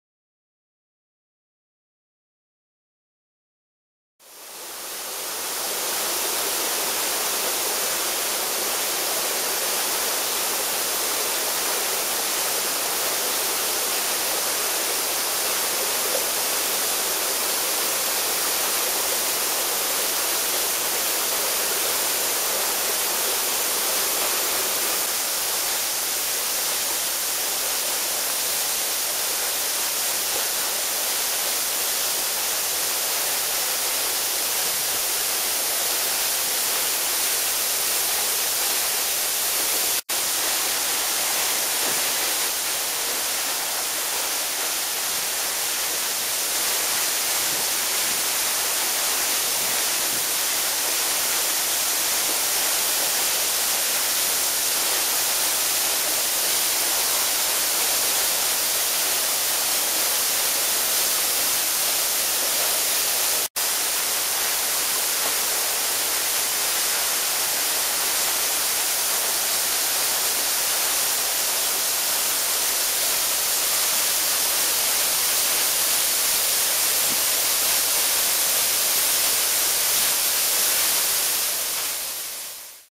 佐賀県唐津市肥前町新古場 白糸の滝
繊細な二条の流形が美しく、玄武岩との対照に特徴がある。